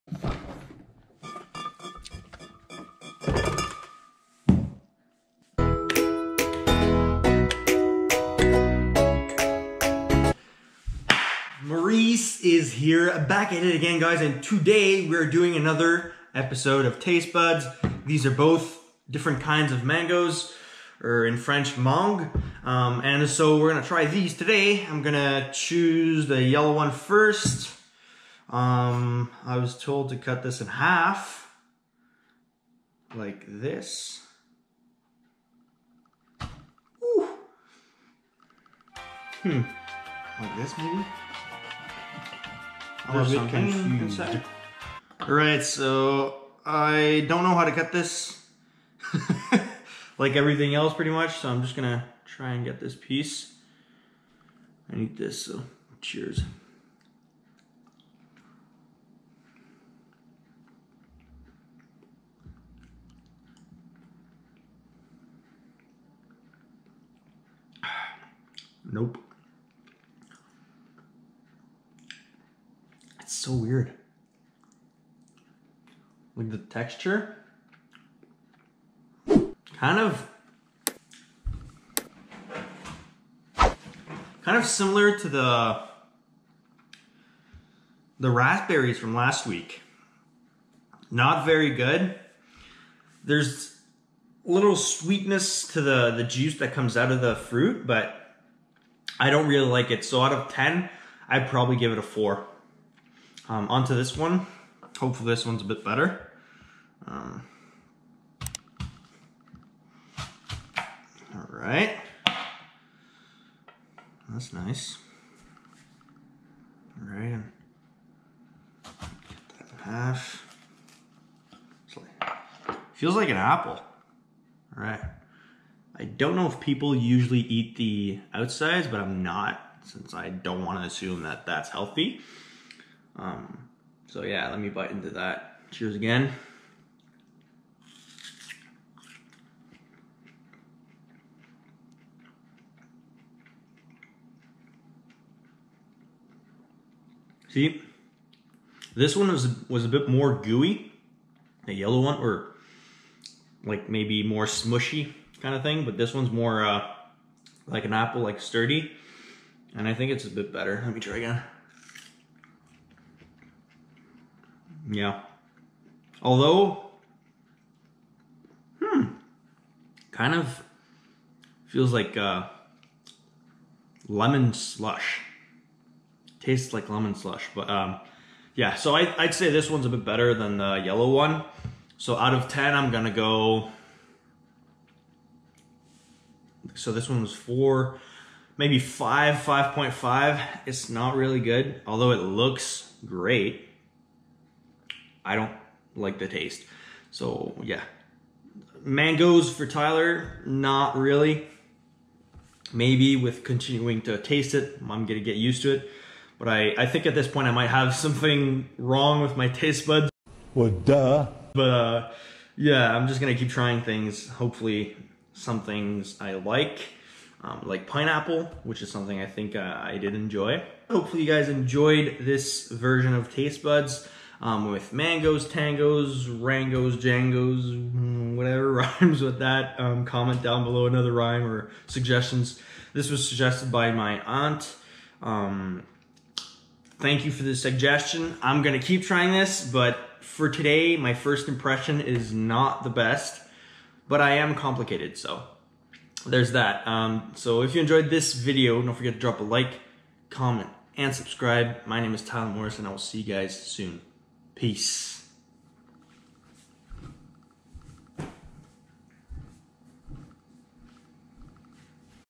Mango Tango | TASTE BUDS Ep.4
Hello everyone!!

As you may know, I don't eat much so I decided to start this series so I can start tasting more stuff and show you the journey! That way we'll be taste buds!

Tyler Morris